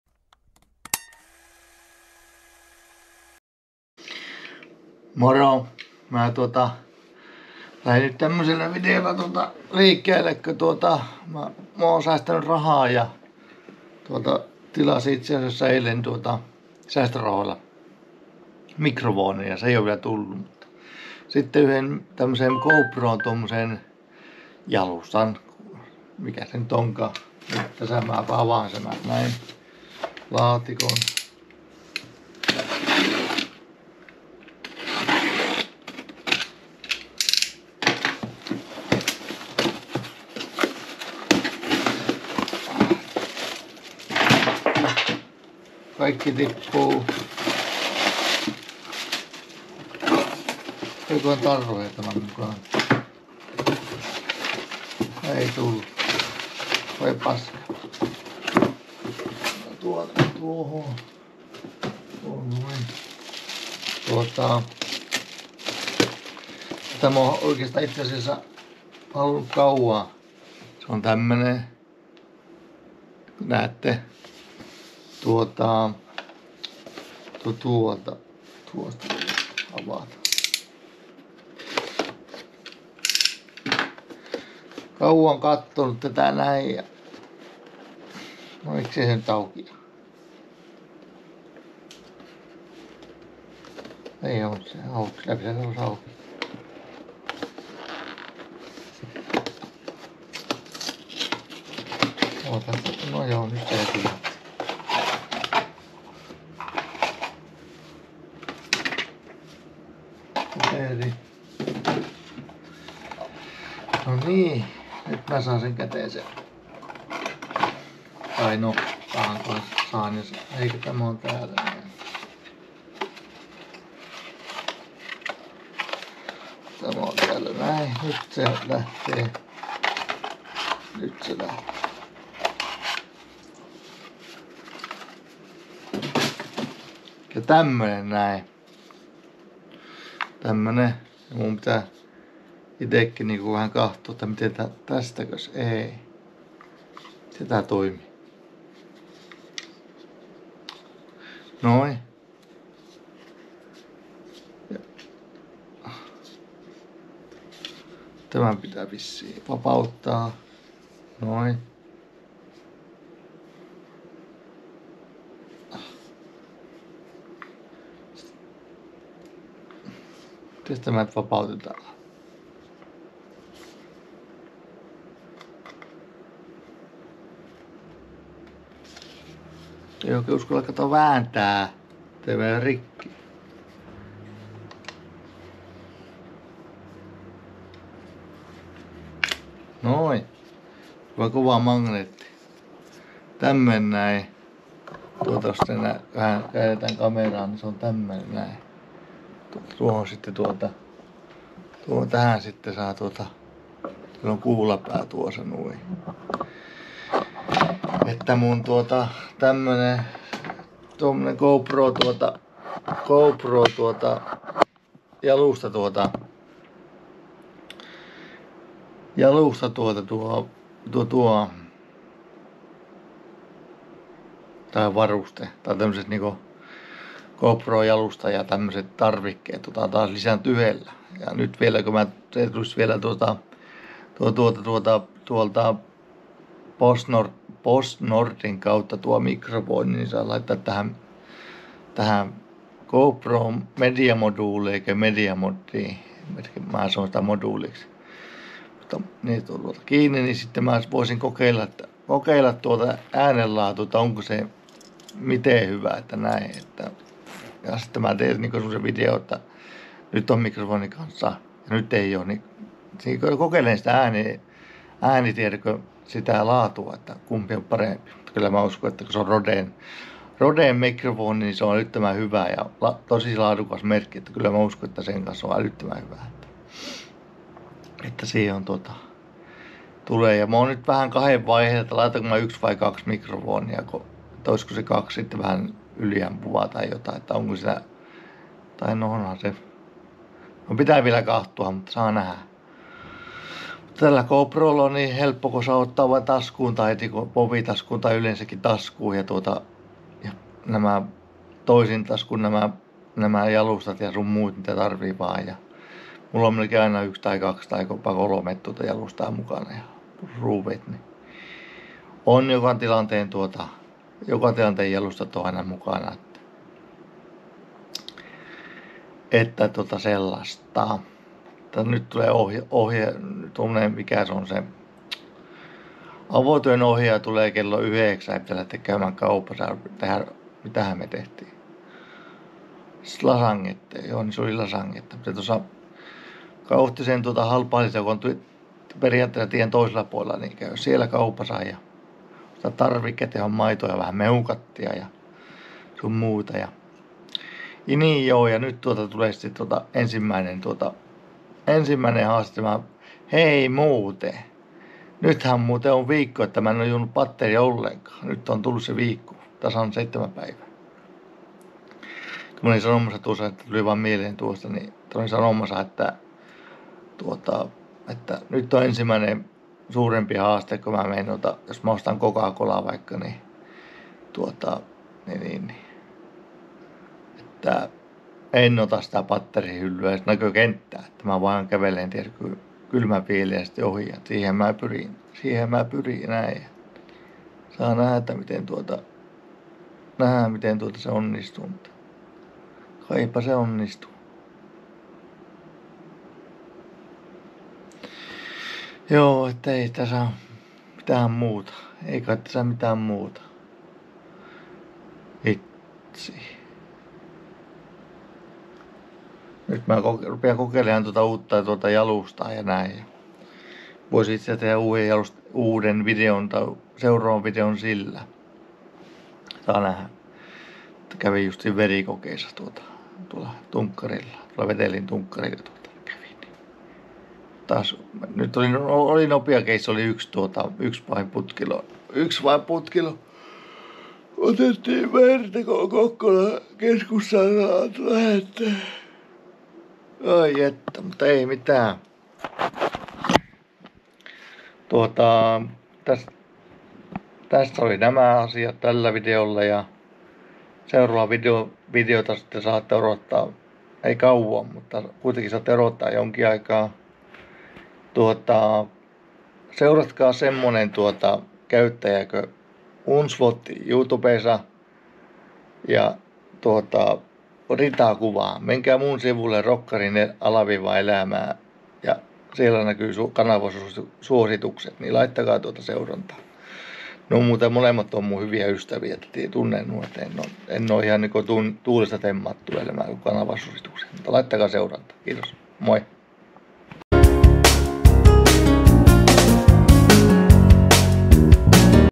GoPro 3-Way Mount 2.0 ja muuta kivaa
Tässä jaksossa siis esittelen minun uuden jalustan ja sitten laittakaa seurantaan sellainen tubettaja kuin Hunsvotti ja Rita kuvaa